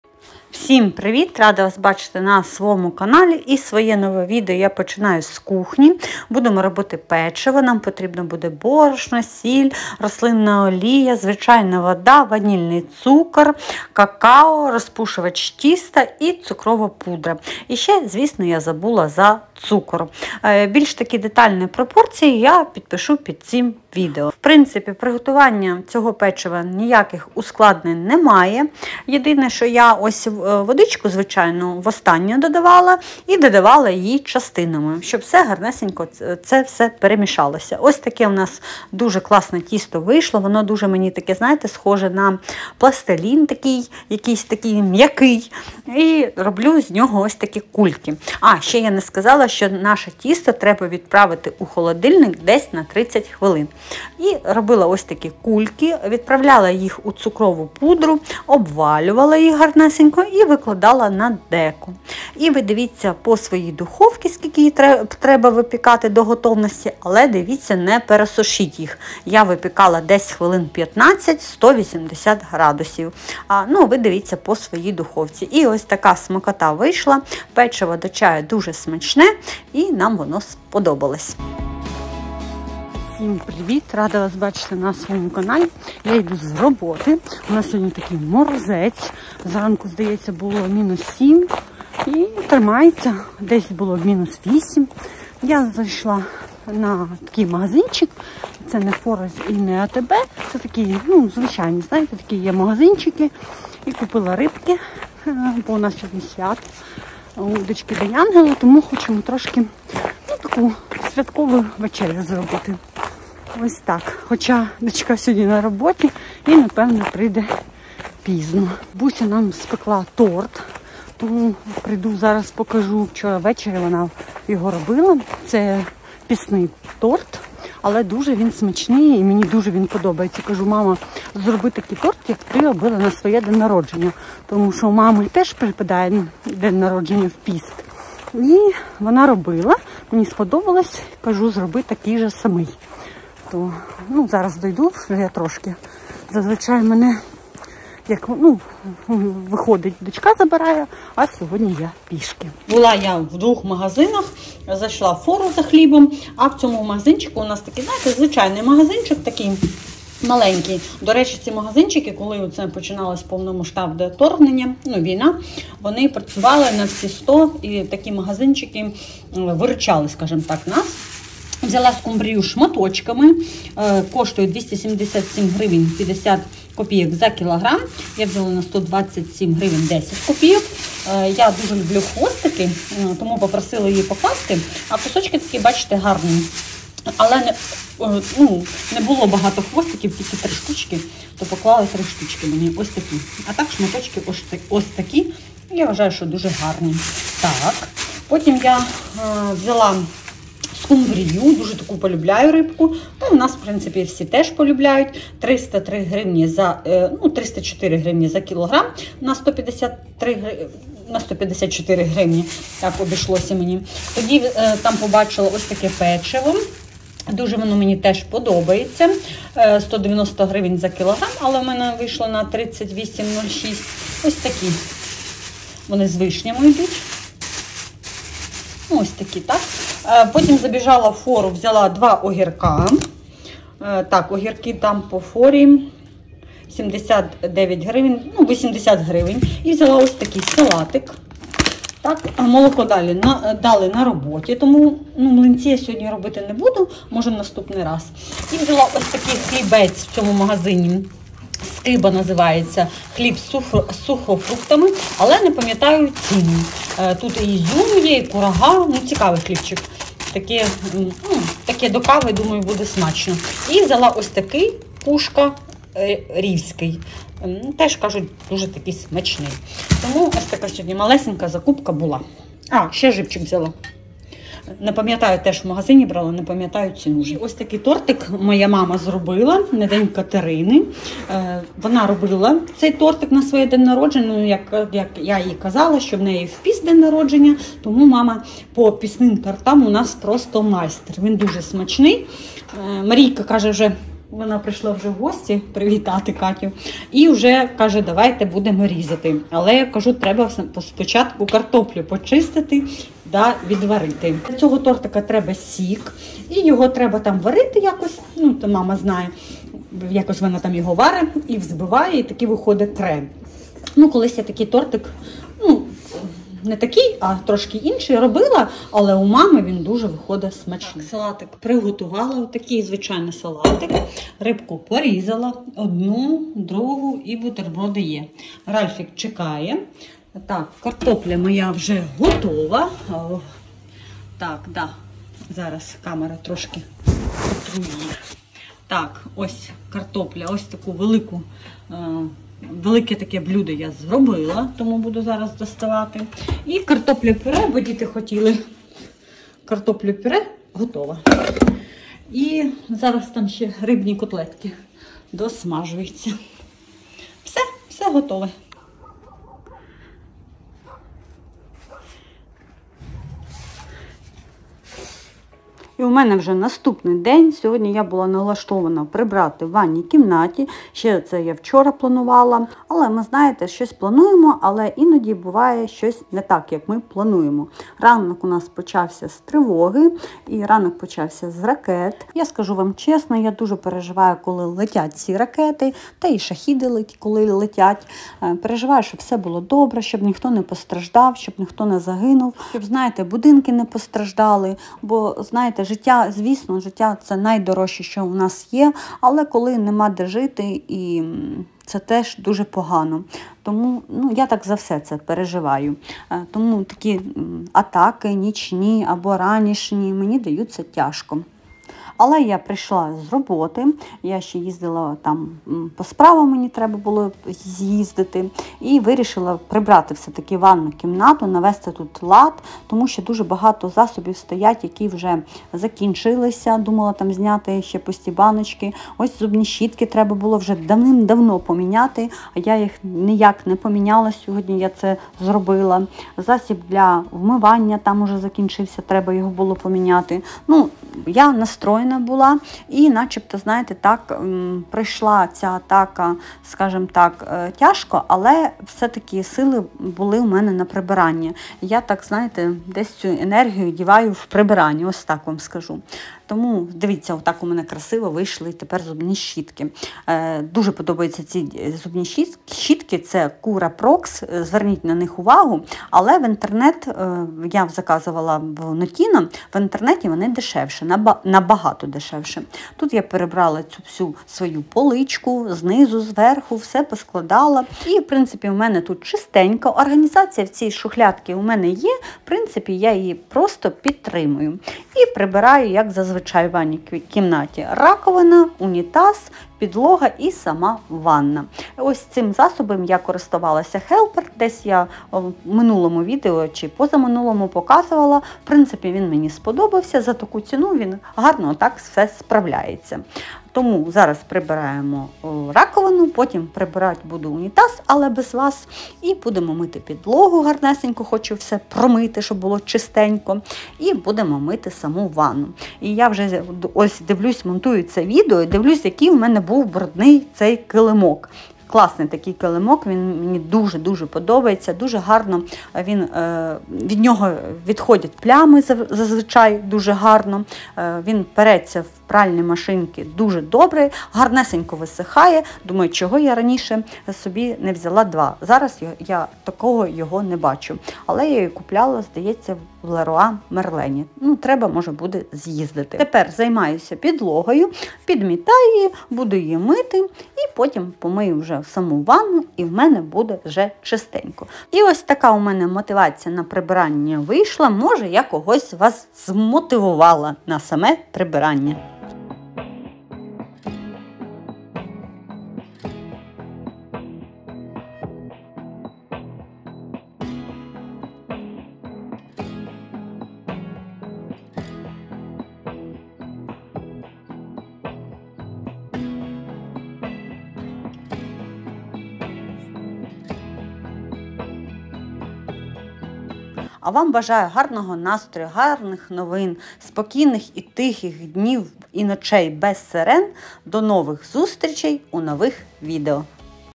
ПІСТНЕ ПЕЧИВО// ЗАКУПКА ПРОДУКТІВ// ТОРТИК НА СВЯТО//МОТИВАЦІЯ
Рецепт печива🍪🍪🍪
100мл води
60 мл рослинної олії
100гр цукру
Дрібка солі
Ванілін або ванільний цукор
60 гр какао
150 гр борошна
Розпушувач тіста
Цукрова пудра